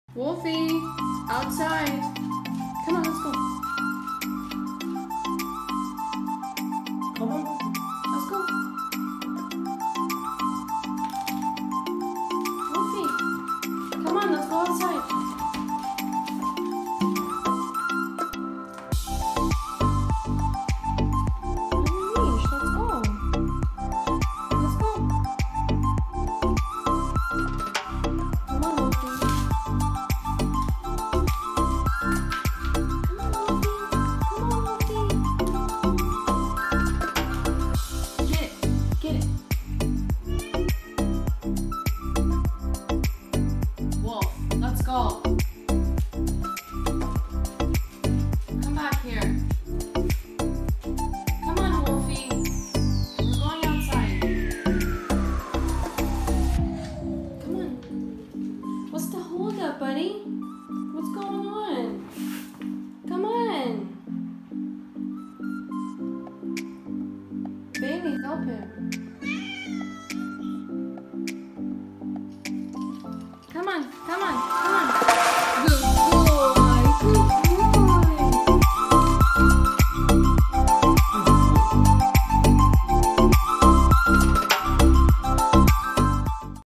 INVISIBLE WALL CHALLENGE WITH MY DOG | LABRADOR CANE CORSO | FUNNY DOG VIDEOS
Musician: @iksonofficial